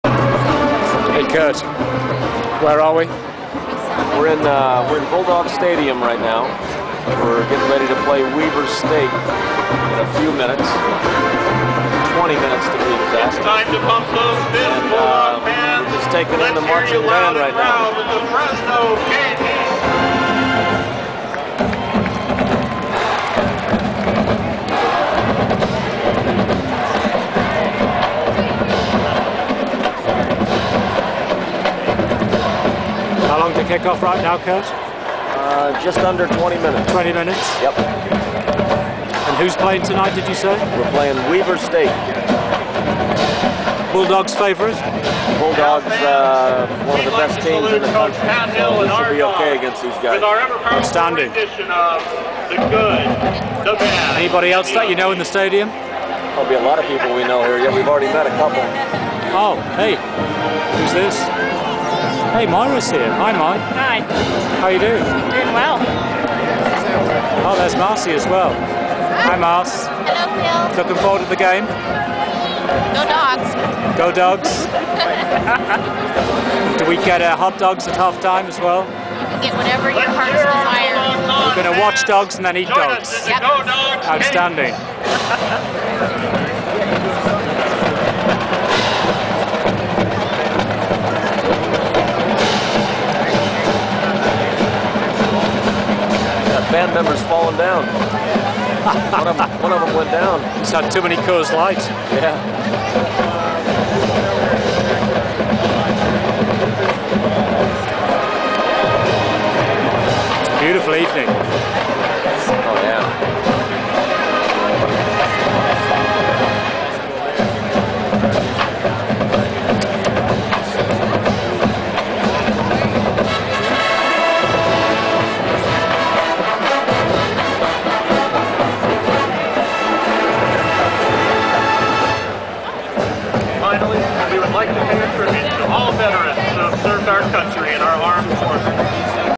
Phil Ging & Kurt Smith at the Bulldog Stadium, Fresno
Phil Ging films a Fresno State football match at the Bulldog Stadium, Fresno with his wife Moira and their good friends Kurt and Marcy Smith.